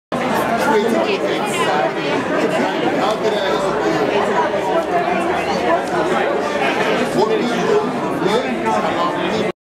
Close up of a sign advertising a job. Job seekers meet po...
Close up of a sign advertising a job. Job seekers meet potential employers at a job fair held in a conference room of the Holiday Inn in Midtown Manhattan.